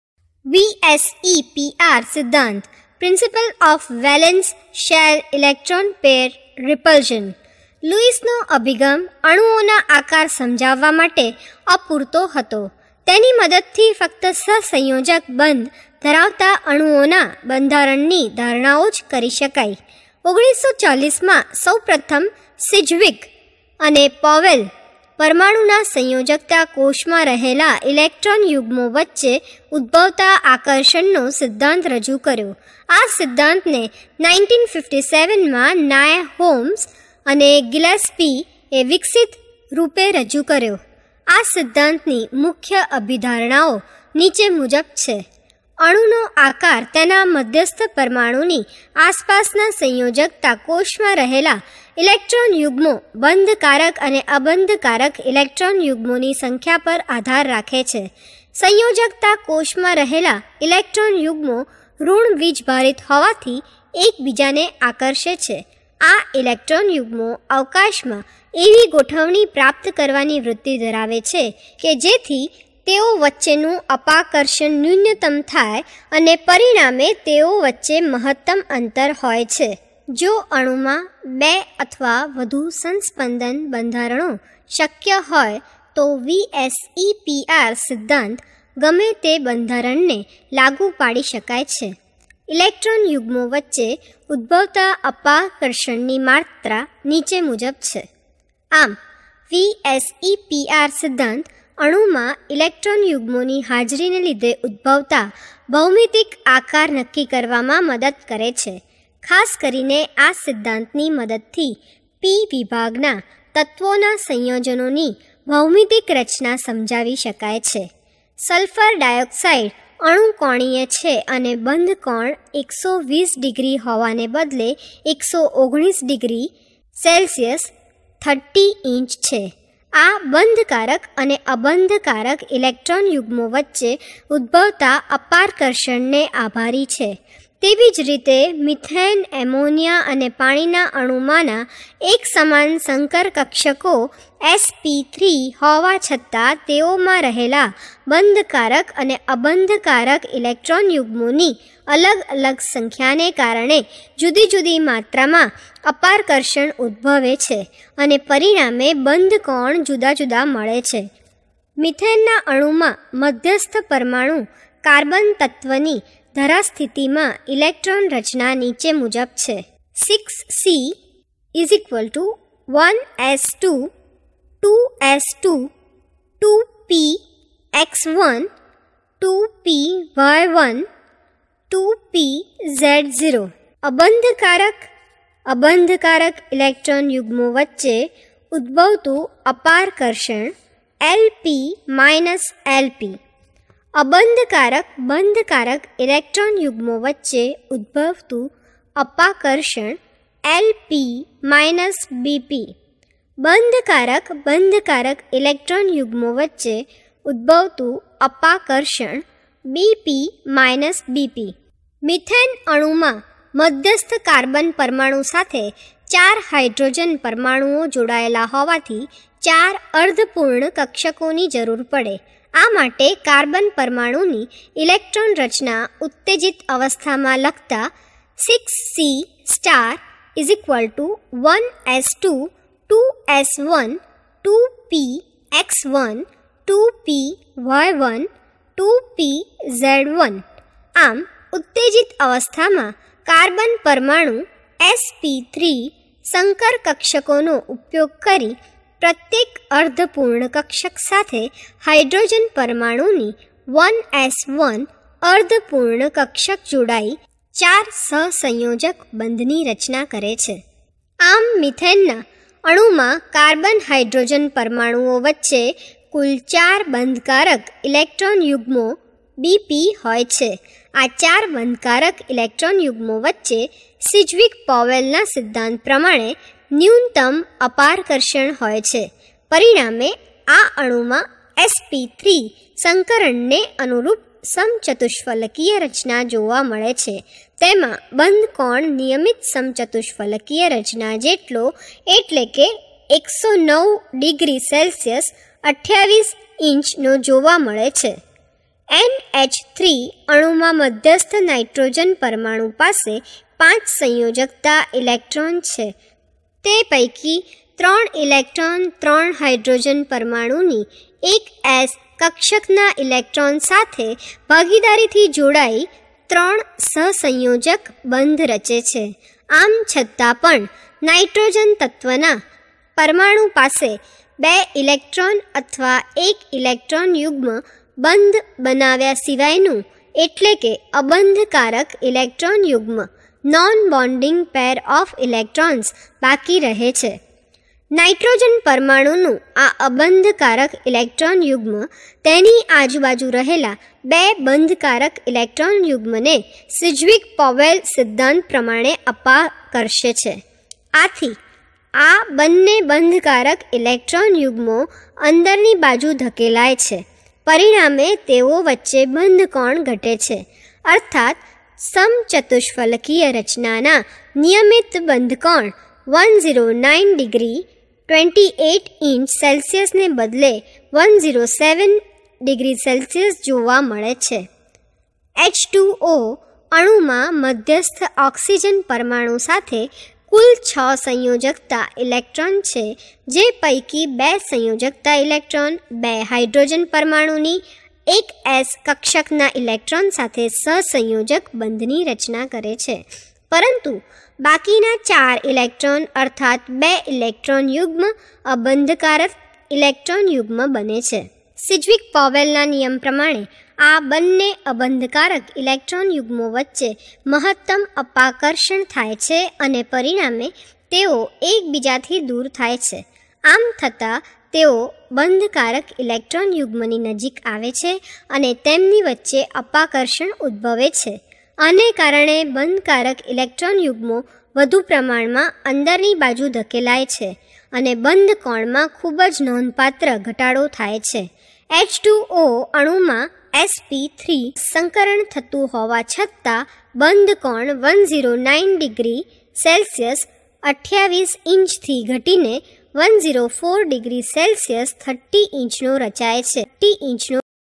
VSEPR principle | VSEPR સિદ્ધાંત | Chemical Bonding and Molecular Structure | 11th science chemistry
VSEPR principle
VSEPR સિદ્ધાંત
Chemical Bonding and Molecular Structure
11th science chemistry

chemical bonding or find difficulty while dealing with Vsepr theory then you are at right place. This visual will help you learn them quick and easy. Please mention your Doubts or Queries in comment section below